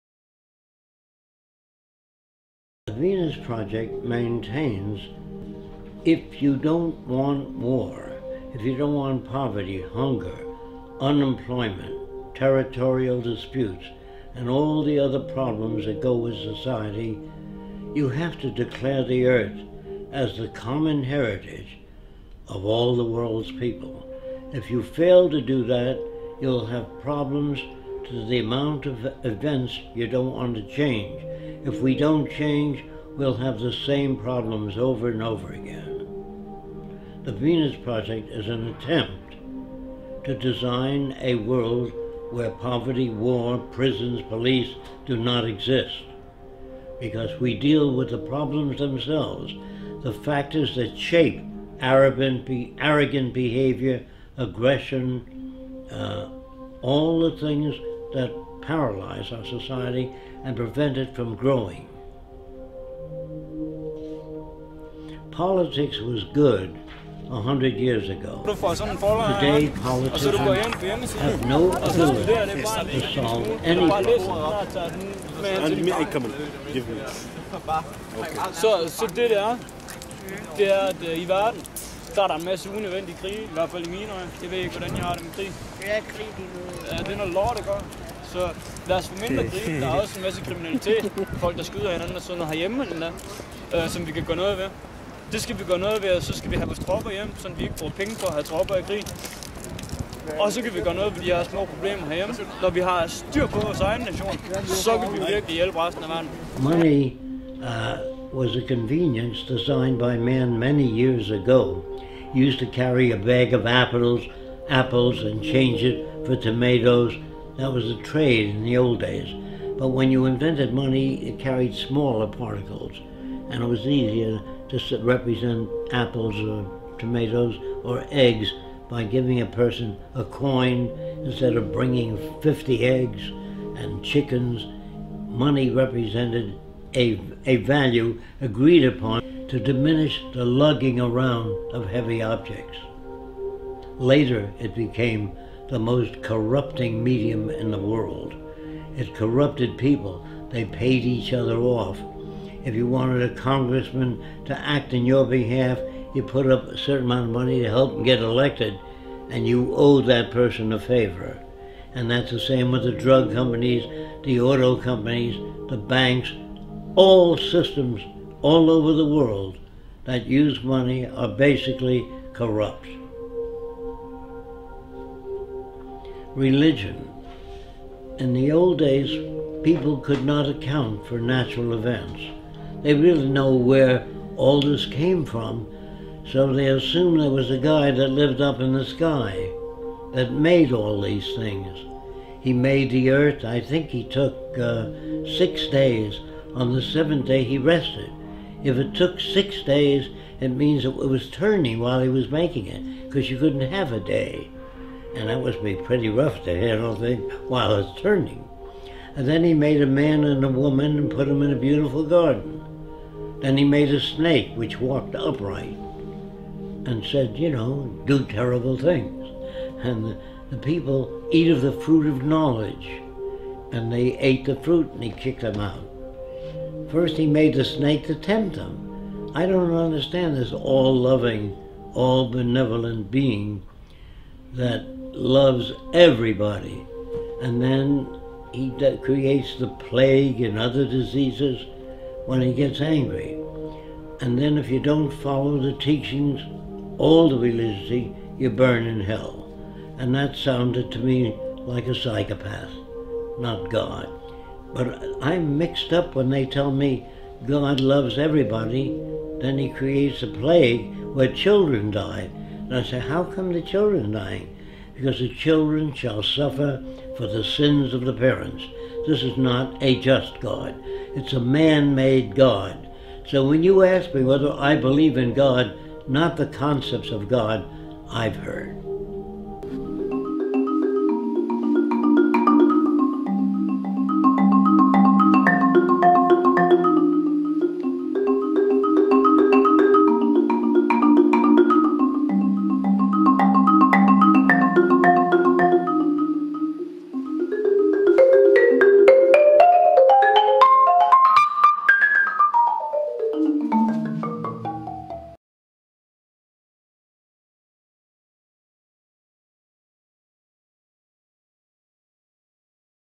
Z day 2010 in Aarhus Denmark 13/03/2010
The Zeitgeist Movement people discussing the Venus Project with a Resource Based Economy, sharing free food with everyone.

Audio track from Earth 2,0 Jacque Fresco Talks
Video track from WhoisPeterJoseph.com

Video Recorded/Edited by
Francoiscamera.webs.com